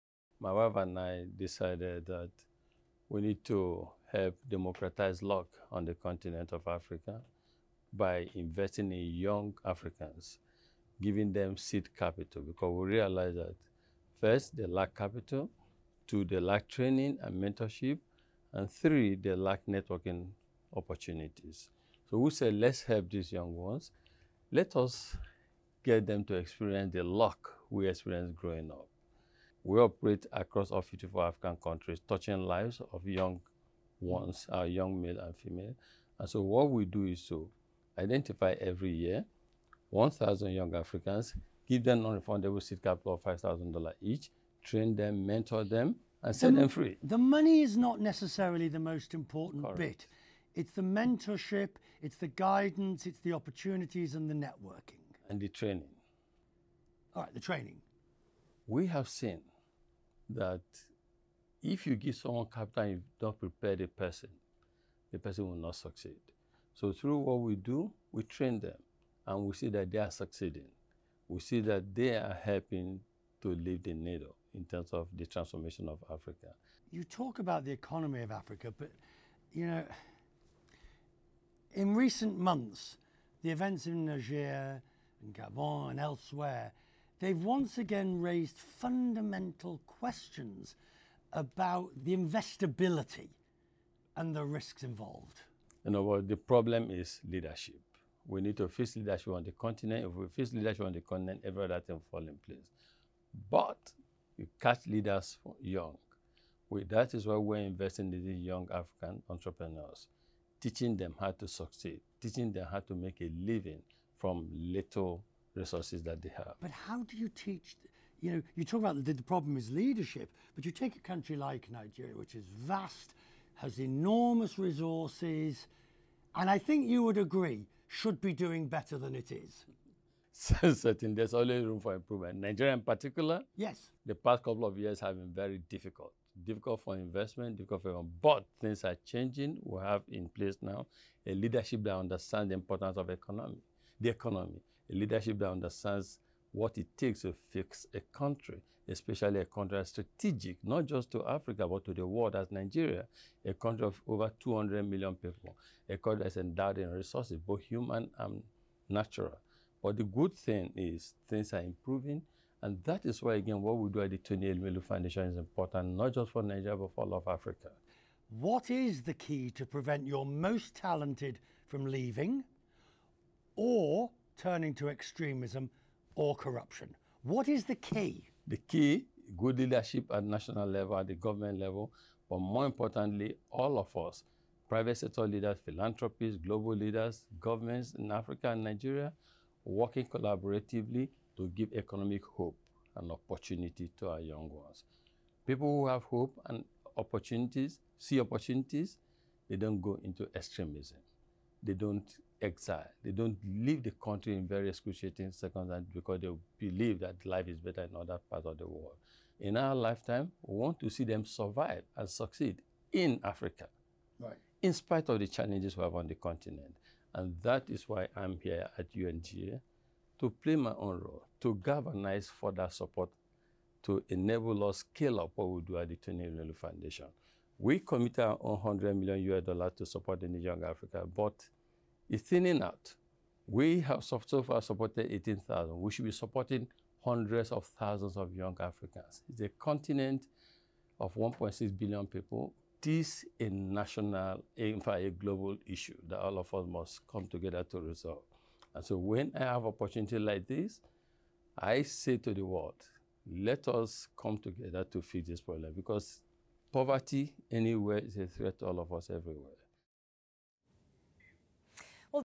Tony Elumelu discusses his foundation with Richard Quest
Leading African investor Tony Elumelu discusses the Tony Elumelu Foundation, which seeks to fund young entrepreneurs on the continent.